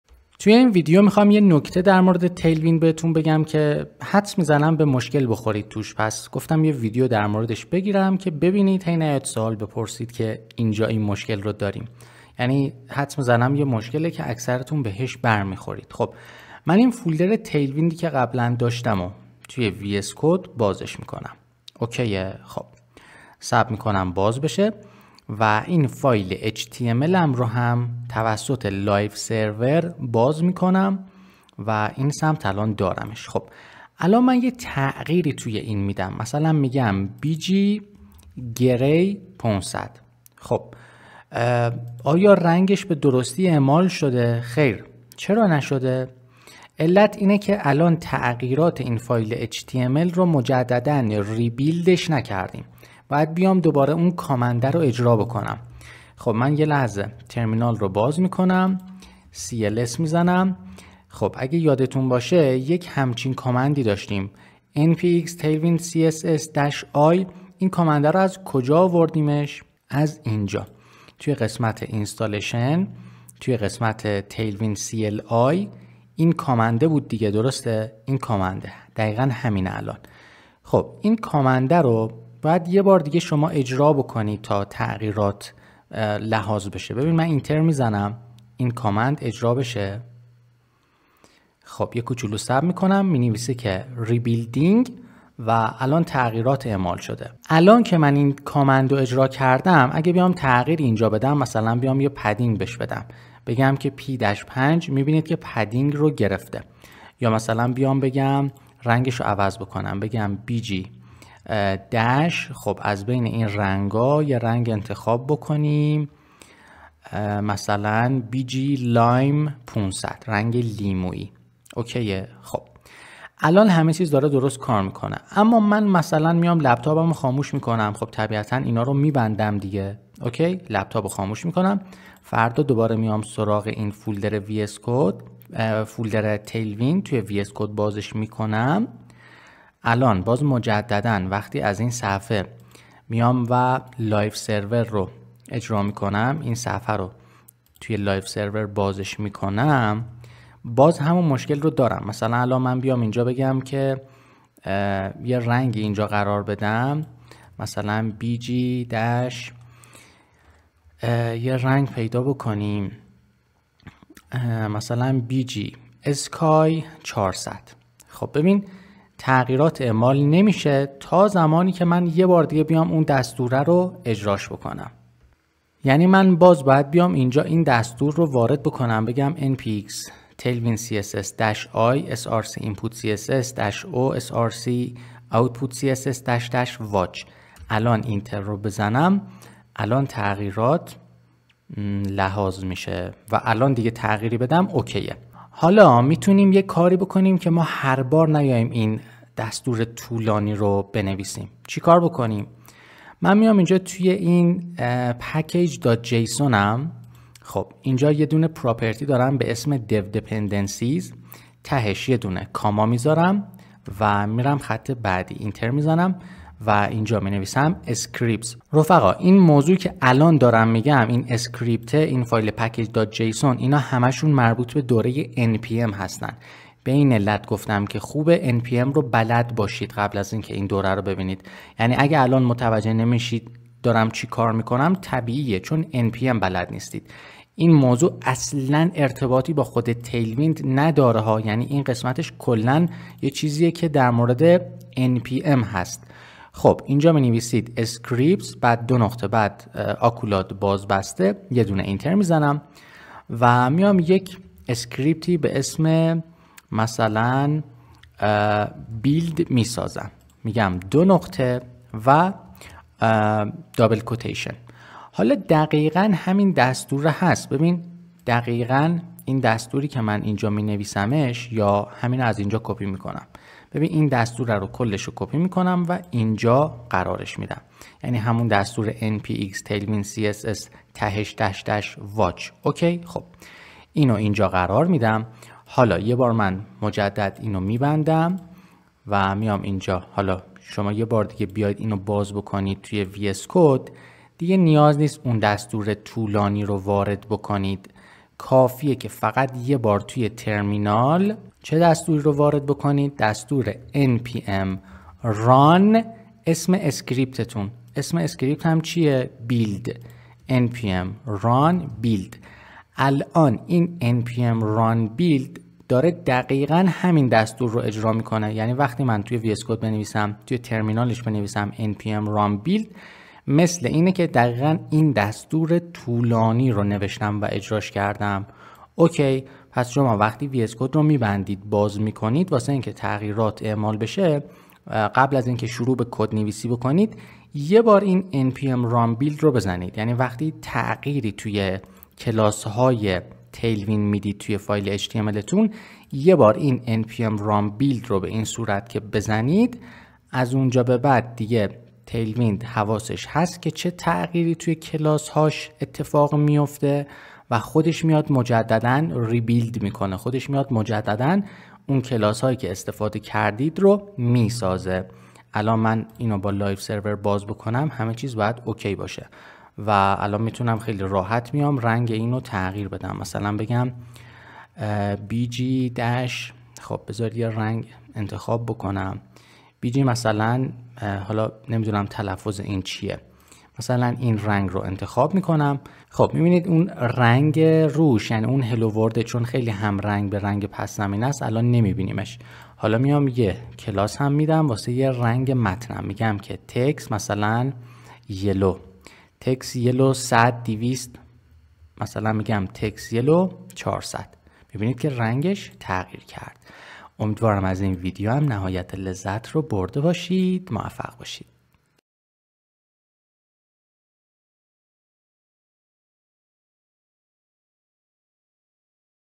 آموزش تیلویند (tailwind css) - ایجاد script در package.json
🔸تیلویند (tailwind css) یک فریمورک Utility First هست ، این فریمورک متشکل از تعداد زیادی کلاس های آماده و از قبل تعریف شده هست که میتونید واسه پیاده سازی هر دیزاینی ازش استفاده کنید.